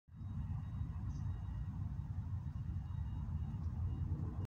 Frozen 4 : Short due to full phone storage, darn it!(CD=See Description) CD-111621
Senior freezes at sight of Jr for a good 30+ seconds. See why it has a green dot nose in video released today. .